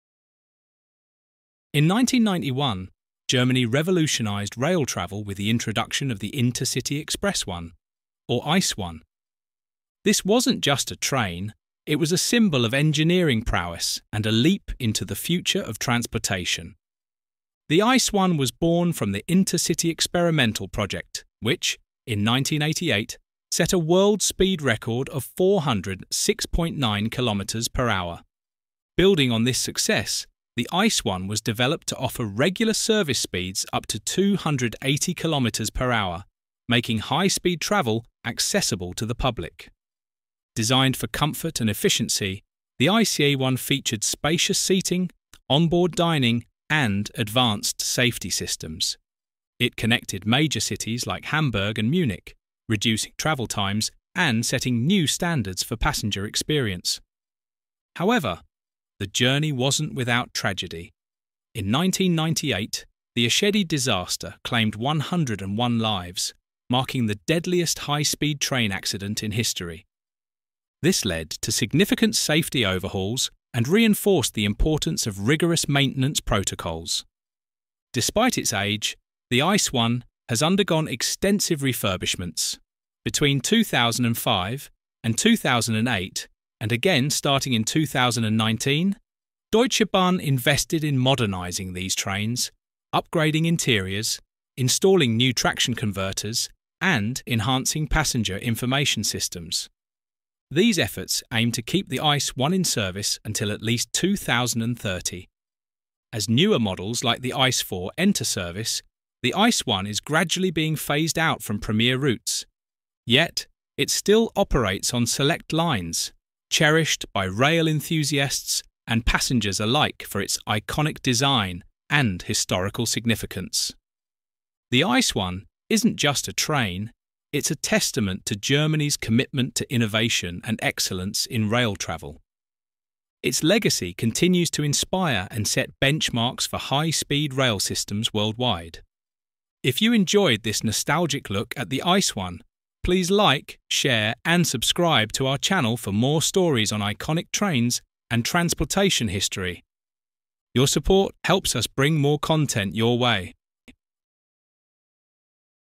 ICE 1: Germany’s High-Speed Pioneer: Revolution or Relic?
🚄 ICE 1: Germany’s High-Speed Pioneer – Was It a Success or Failure?

Description:
Step aboard the legendary ICE 1, Germany's very first high-speed train, and embark on a journey through engineering innovation, triumph, tragedy, and legacy. 🇩🇪🚆

In this video, we take you deep into the incredible story of the Intercity-Express 1, launched in 1991 as a symbol of a new era for Deutsche Bahn and German rail travel. Built from the dreams of the experimental ICE-V that shattered speed records, the ICE 1 promised a future where distances between major cities like Hamburg, Frankfurt, and Munich would shrink dramatically.

🔹 How was the ICE 1 conceived and developed?
🔹 What role did it play in transforming Germany’s rail network?
🔹 How did the tragic Eschede disaster in 1998 change rail safety forever?
🔹 Was the ICE 1 ultimately a success or a failure?
🔹 Where can you still ride the ICE 1 today, and how is it being replaced?

We’ll explore how the ICE 1 revolutionized passenger comfort with spacious cabins, dining cars, and speed capabilities that set new standards at the time. Despite facing challenges, it earned a place in the hearts of travelers and remains a nostalgic icon for rail enthusiasts around the world.

Even today, many refurbished ICE 1 sets are still in service, albeit slowly being phased out by the newer ICE 3 and ICE 4 models. Yet, the story of the ICE 1 continues to influence train design and rail networks worldwide.

This is more than just a train — it’s a time machine back to when rail travel took a giant leap into the future. 🌍✨

👉 If you're a fan of trains, technology, or European history, you won’t want to miss this in-depth tribute!